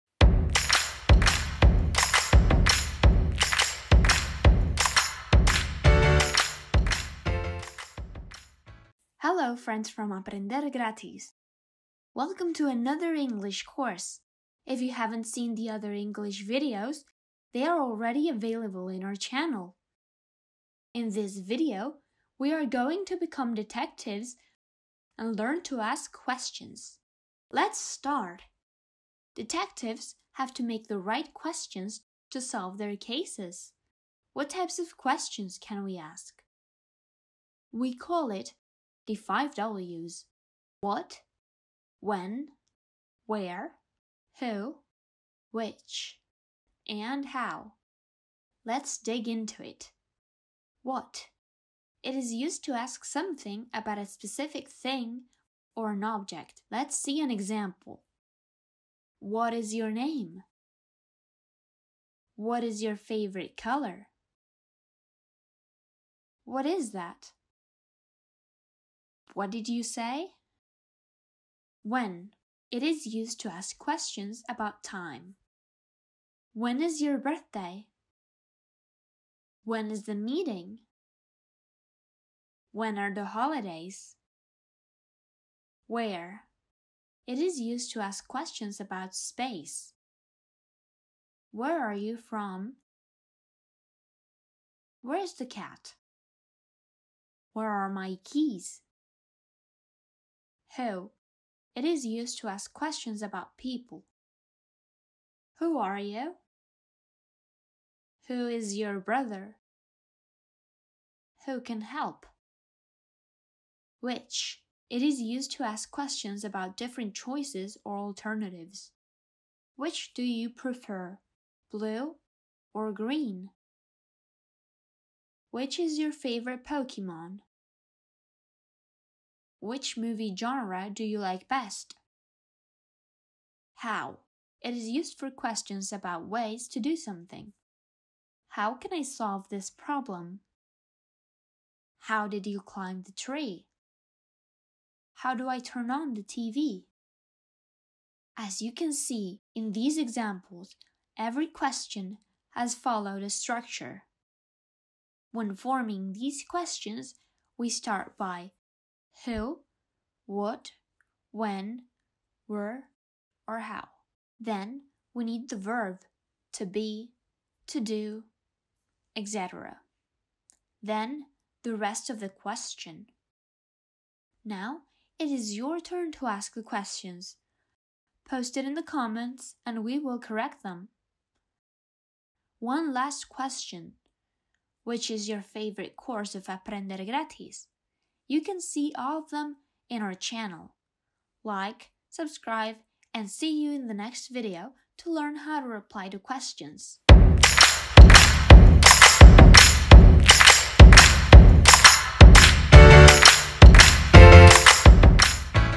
Las 5 PREGUNTAS en INGLÉS (Question words) CURSO INGLÉS FÁCIL
¡Hola! Te damos la bienvenida a un nuevo curso de Inglés Fácil de Aprender Gratis.

En este primer vídeo verás las diferentes formas de realizar preguntas en inglés. Conocerás las 5Ws: who, what, when, where, which, how y cómo utilizarlas.

00:00 Bienvenida al Curso de Inglés Fácil.
00:28  Qué tipo de preguntas podemos hacer en inglés?
00:39 5Ws en inglés
00:50 Cuándo se usa la pregunta WHAT?
01:11 Usar la pregunta WHEN?
01:25 Cuándo se usa la pregunta WHERE?
01:40 Cuándo se usa la pregunta WHO?
01:53 Cuándo se usa la pregunta WHICH?
02:13 Cuándo se usa la pregunta HOW?
02:28 Estructura de las preguntas en inglés
02:53 ¡Ahora es tu turno de hacer preguntas! Publícalo en los comentarios y los corregiremos.